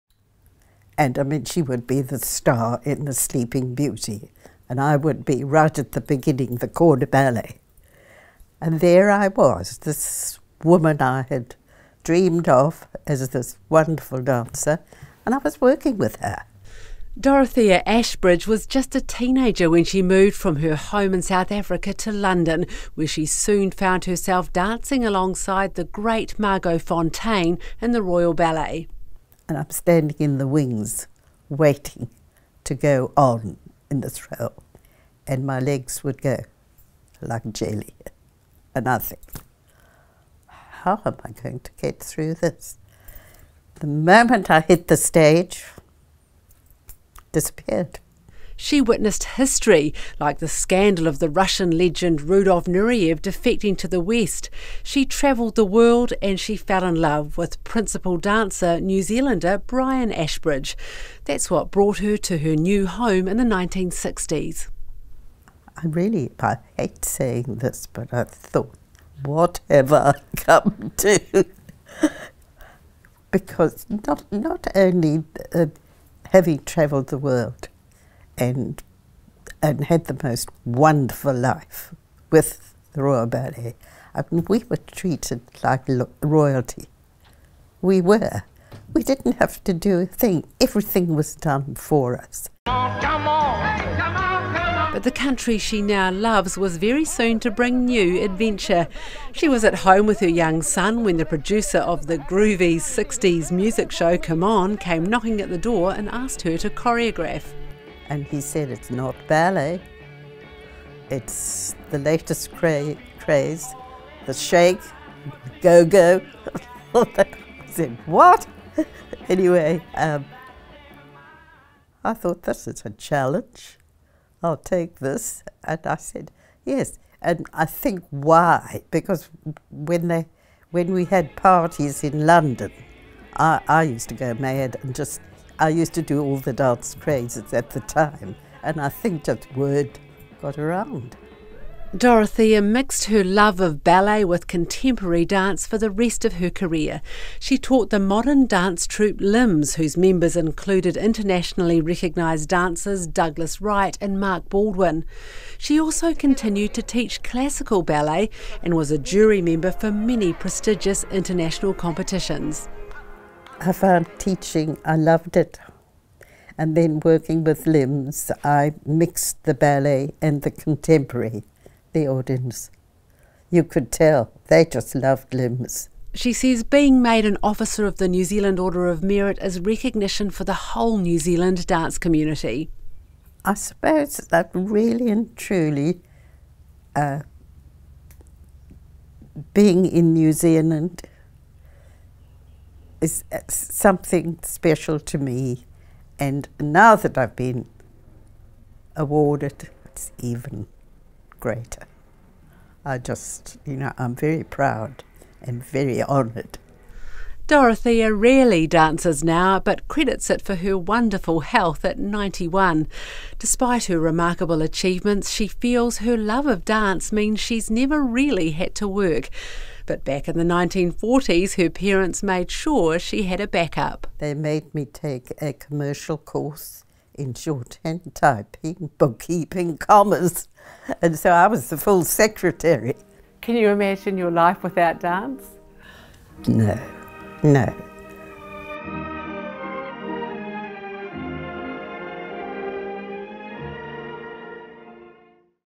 World-class dancer Dorothea Ashbridge made Officer of NZ Order of Merit
Dorothea Ashbridge,  who performed with the world's greatest ballet dancers before teaching go-go girls to shake and shimmy on New Zealand TV, has been made an Officer of the New Zealand Order of Merit.